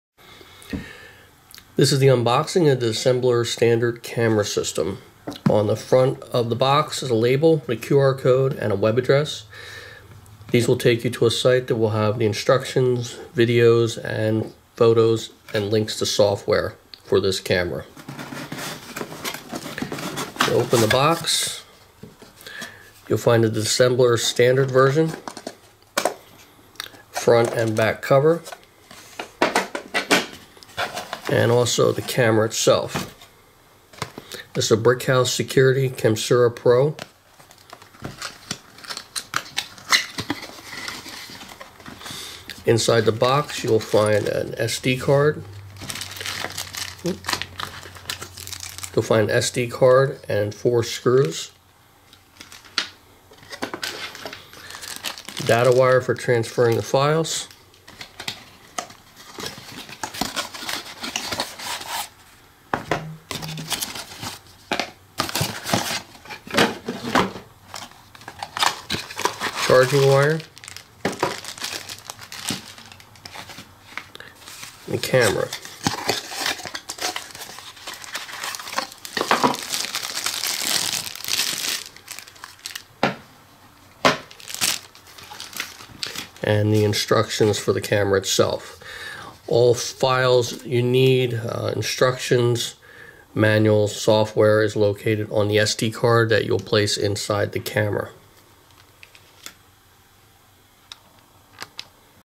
Unboxing Dissembler Standard Camera System
Unboxing of the Dissembler Standard Camera System. Covert Camera system for use by Law Enforcement and Private Investigators.